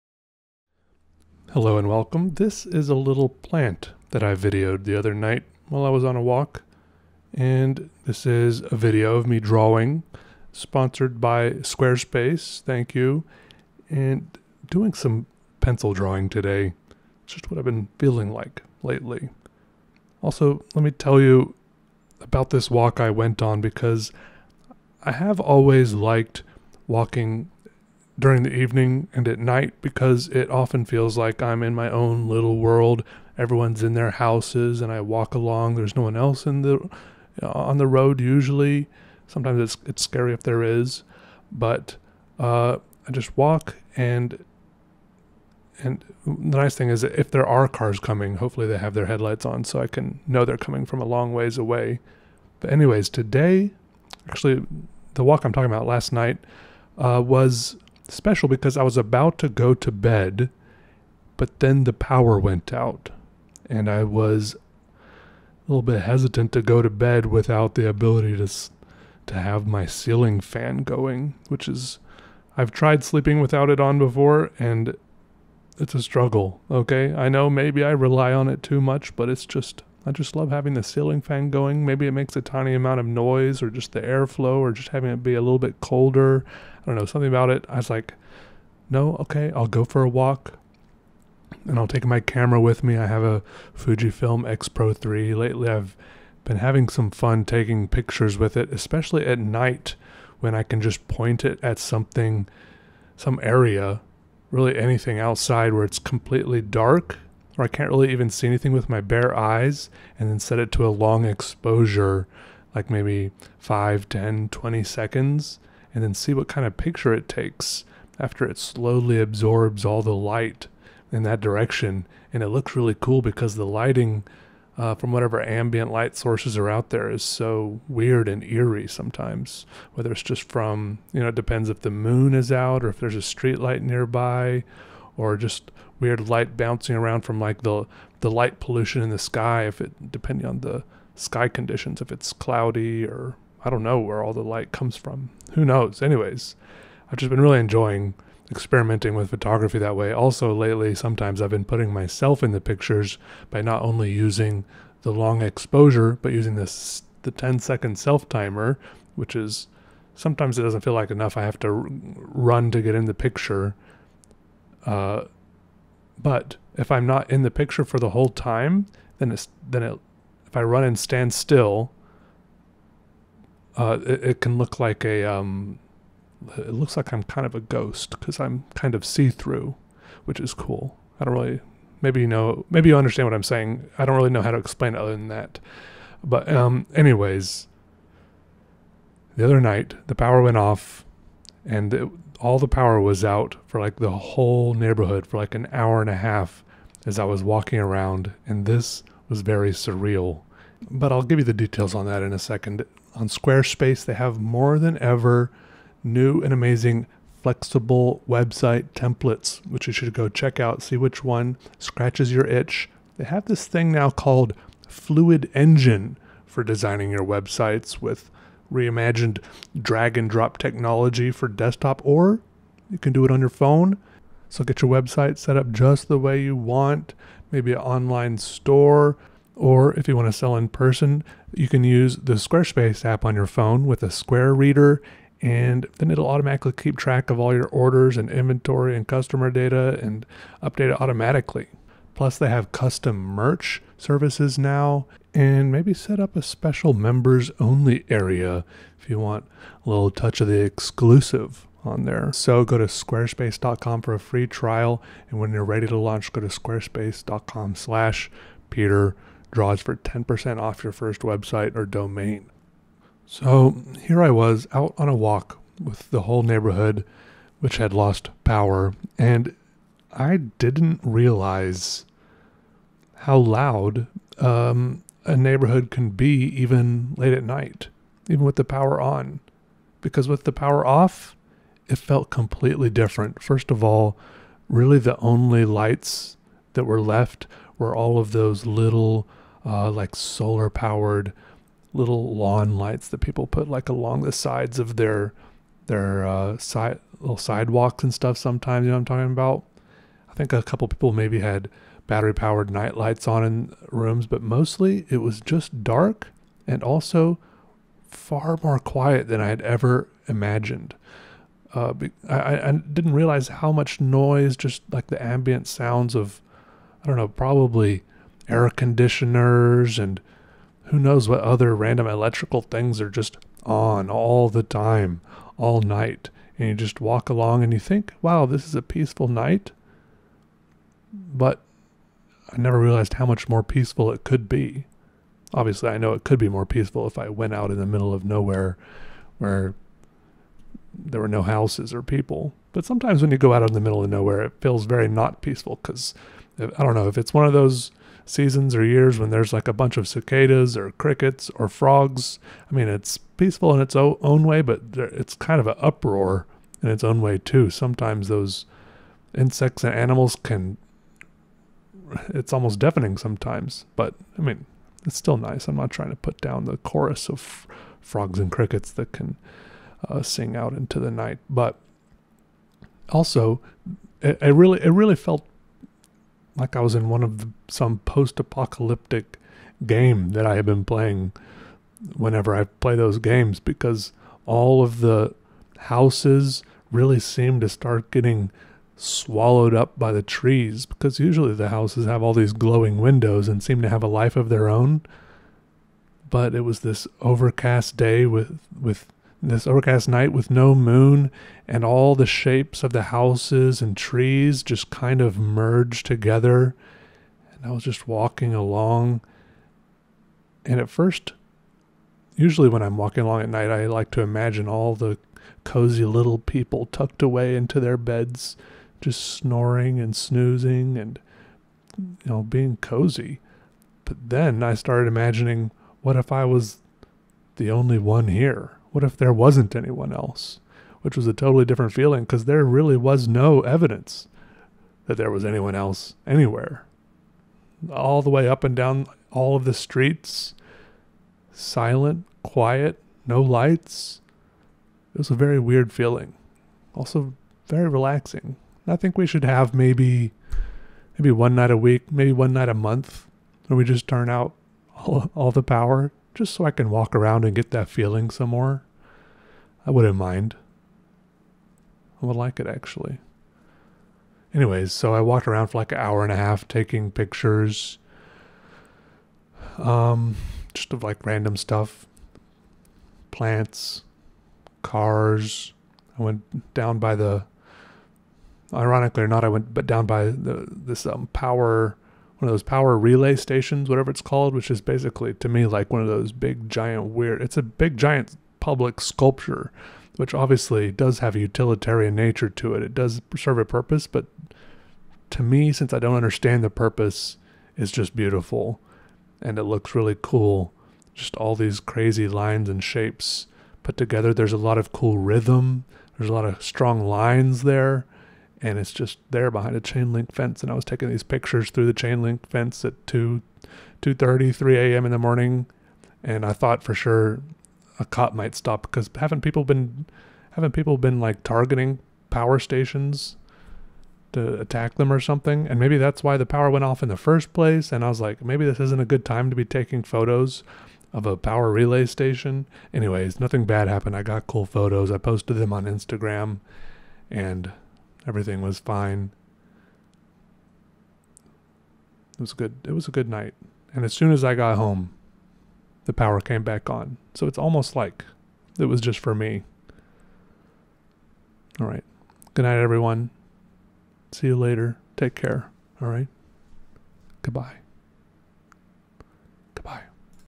A Careful Explosion of Graphite (Pencil Drawing)
Here I used Mitsu-Bishi drawing pencils, a few different hardnesses, just scribbling and sketching on some cardstock, basically thick printer paper. It's nice because it has a little bit of tooth.

My new book Dayfever
The rest of my books